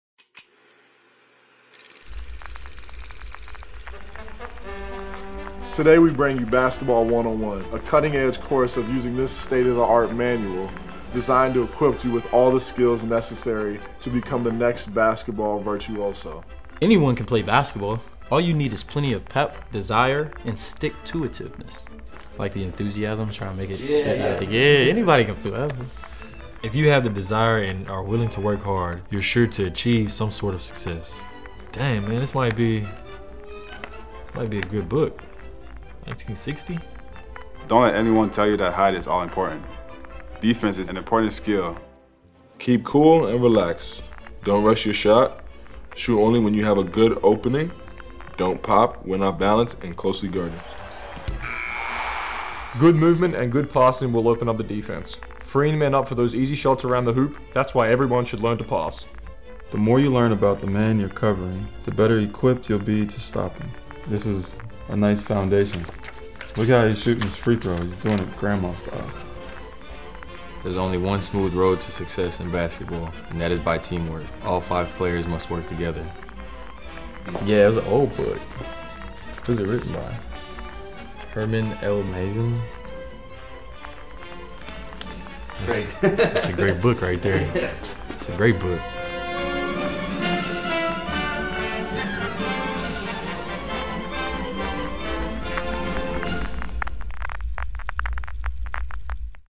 "How To Star In Basketball" with Shai, Josh, Lu, Aaron, Kenrich, JRE, Lindy & Baze | OKC Thunder
Want to learn the fundamentals of basketball to become a star? Shai Gilgeous-Alexander, Josh Giddey, Lu Dort, Aaron Wiggins, Kenrich Williams, Darius Bazley, Lindy Waters III and Jeremiah Robinson-Earl each share key fundamentals and skills needed to become a star in basketball based on the 1960's book "How To Star In Basketball" by Herman Masin.